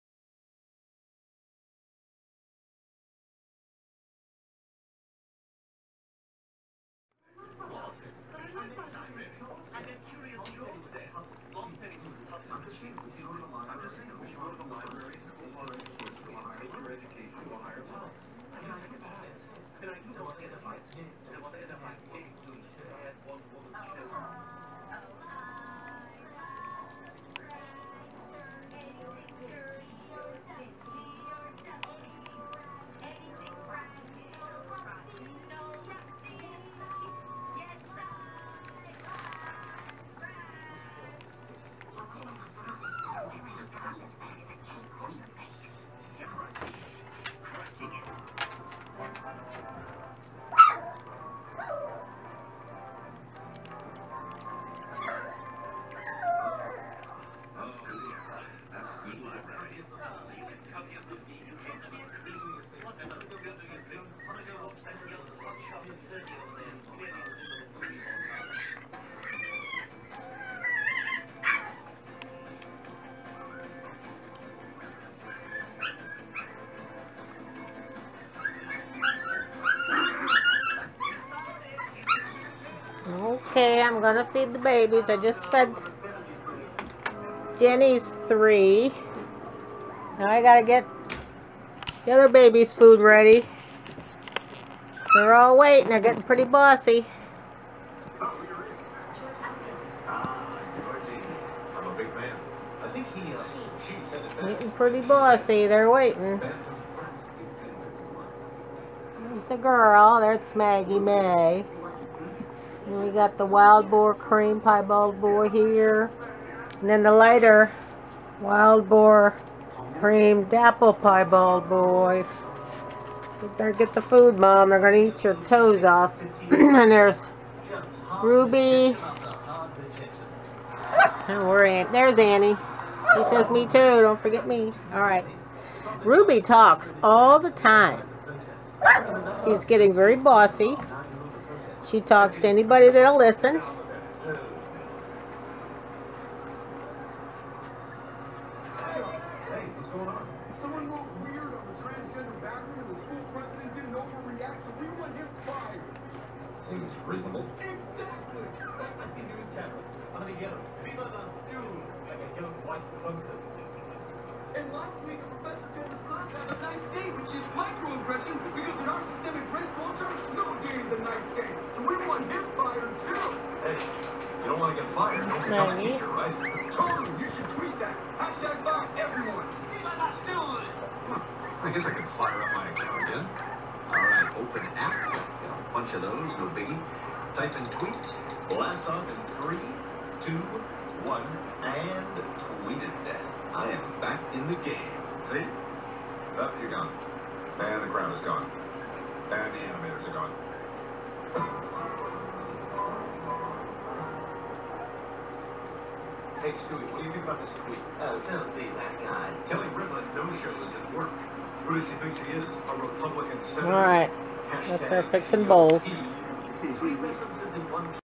Jennee's 3 females first cottage cheese supper
Also ~ Jessee's 3 Wirehair pups & Ruby & Annee